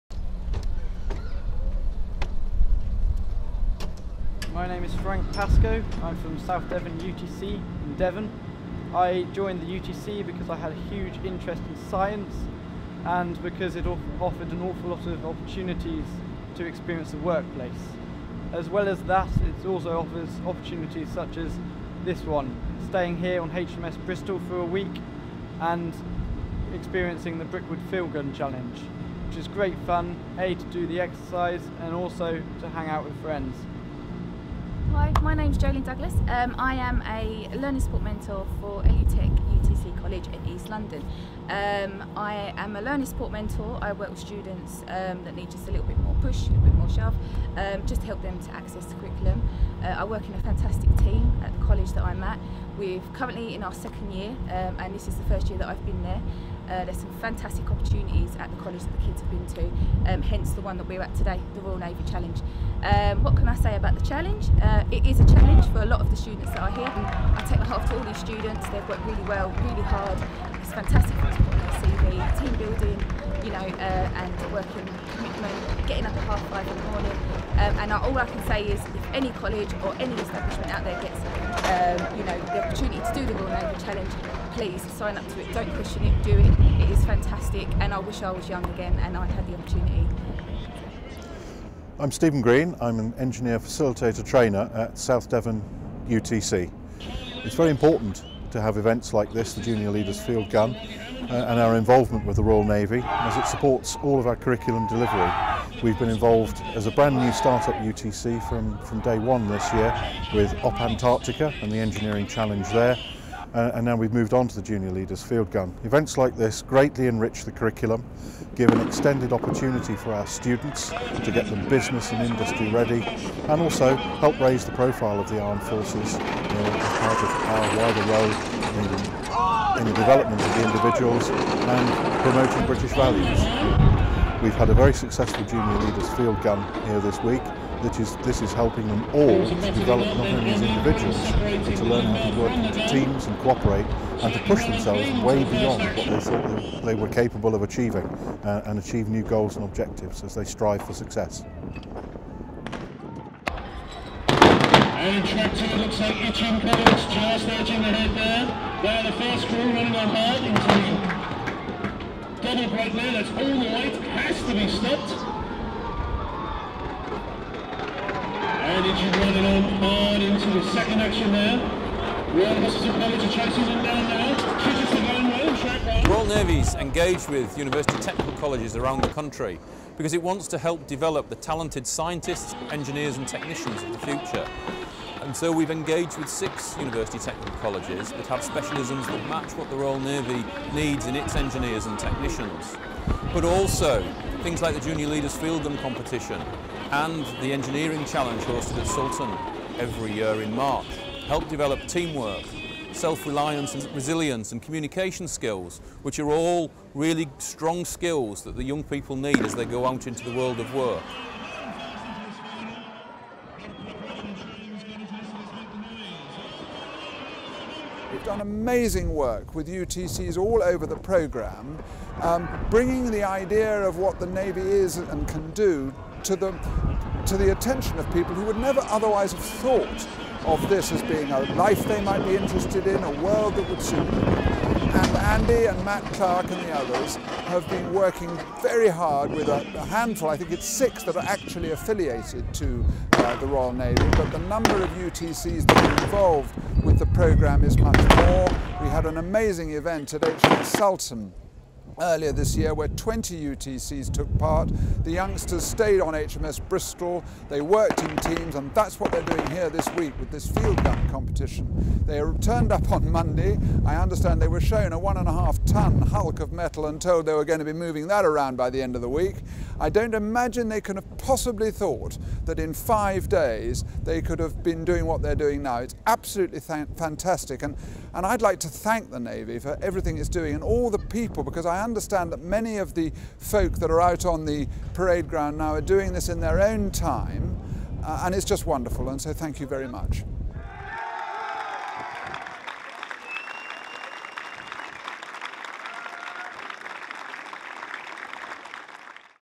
Royal Navy Junior Leaders Field Gun Challenge and UTCs
Watch this Royal Navy production on the Junior Leaders Field Gun Challenge 2016, a national competition involving UTCs, schools, colleges and Royal Navy apprentices.

This short film features UTC students and mentors; Captain Andy Cree, UTC Team Leader and Captain for Royal Navy, and Charles Parker, CEO for Baker Dearing Educational Trust, the organisation that supports and promotes University Technical Colleges. It provides insights into the challenge experience, why it was set up and the benefits for all involved.

Six UTCs have been involved in the challenge so far, including Elutec UTC, South Devon UTC, South Wiltshire UTC, Energy Coast UTC, and Derby Manufacturing UTC. These have specialisms  that match what Royal Navy looks for in its engineers and technicians.